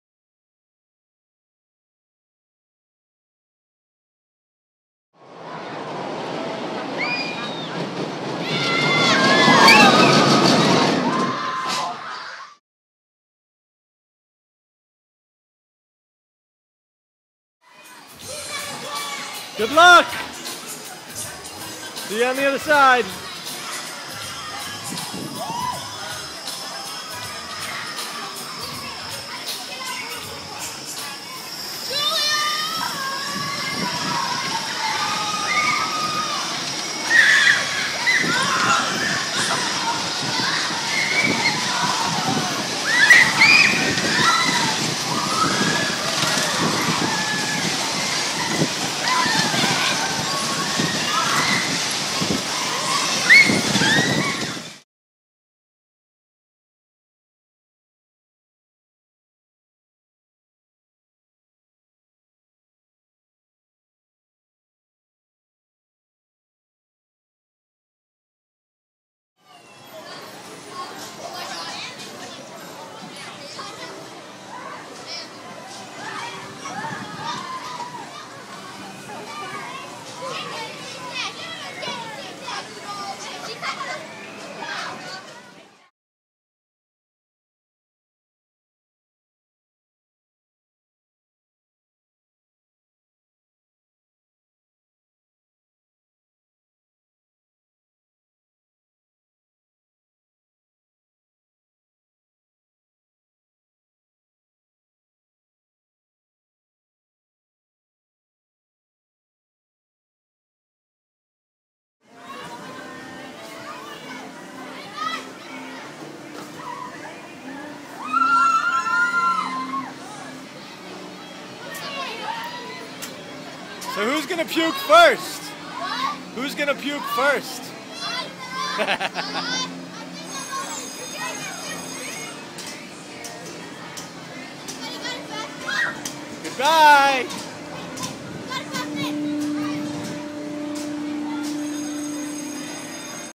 5th Grade Adventureland Field Trip
Kaitlyn's last fifth grade field trip.